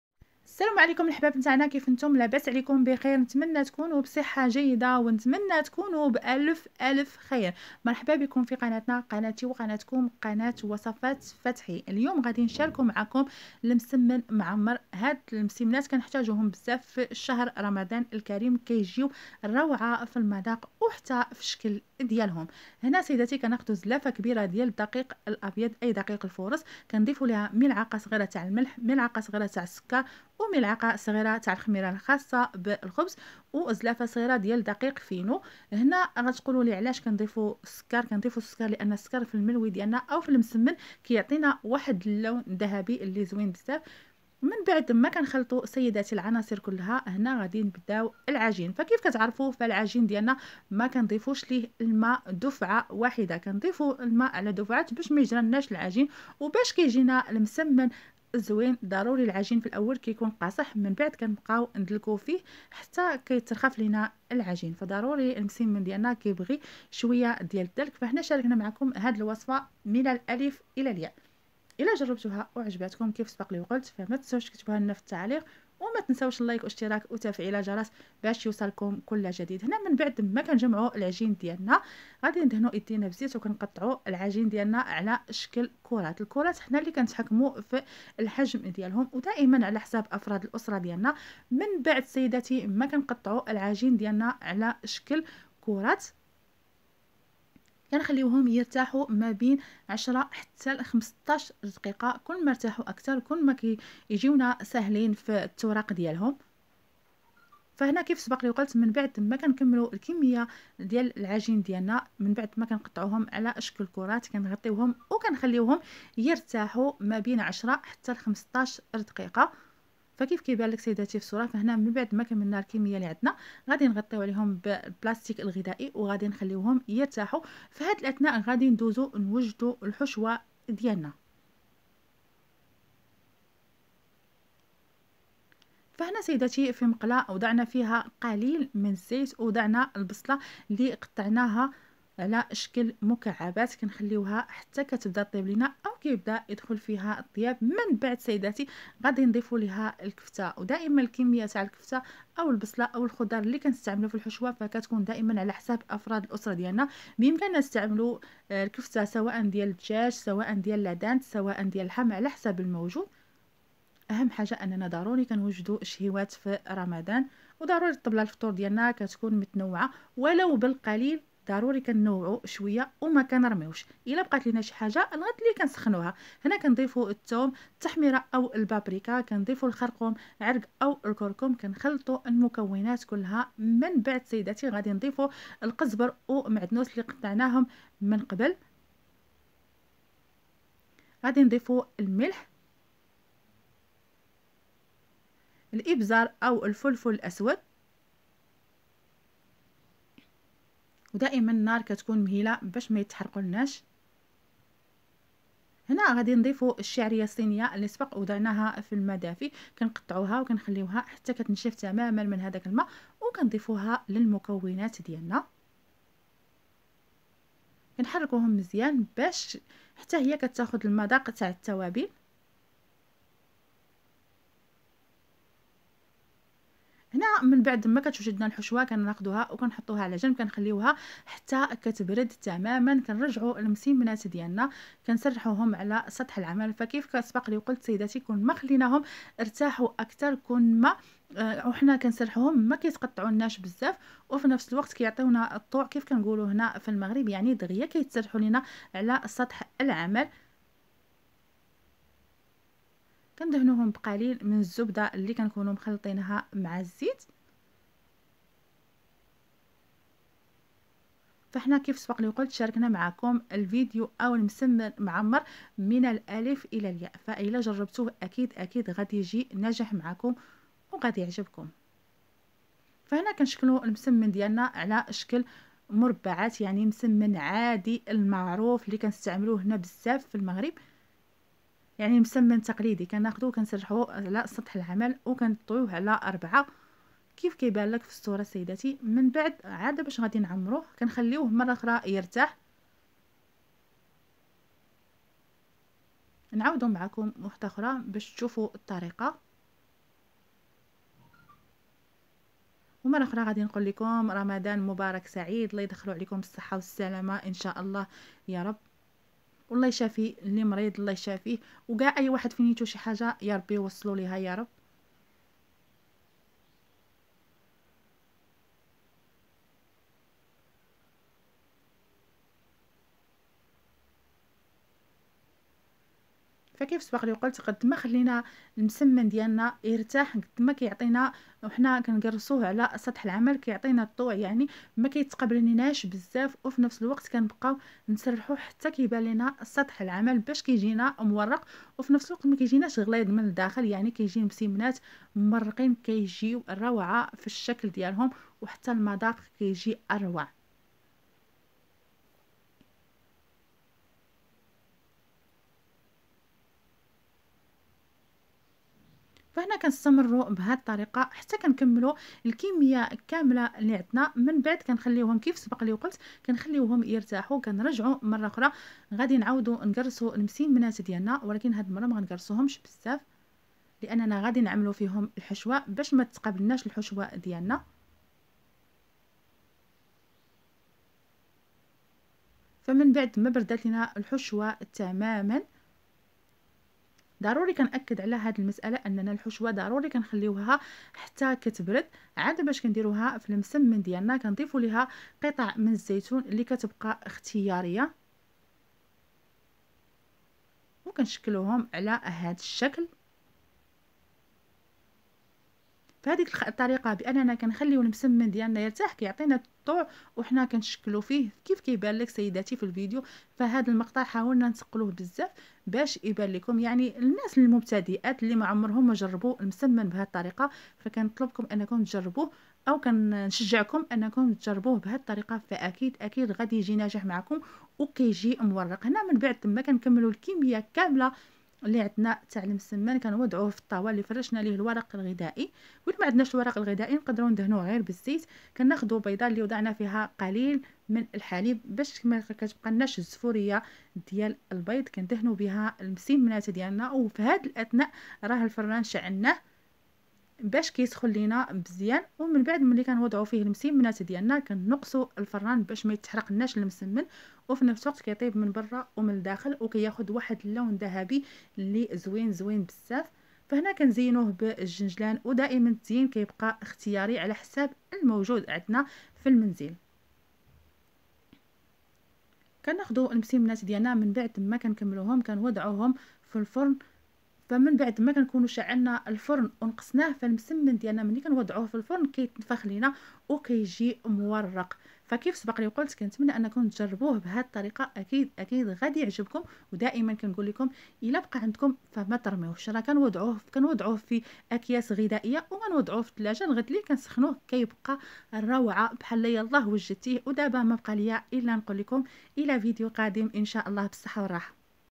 تحضيرات رمضان الذ مسمنات بحشوة روعة في المذاق والشكل وصفات فتحي #2023
رمضان كريم
                                        # لاتنسو اننا نحبكم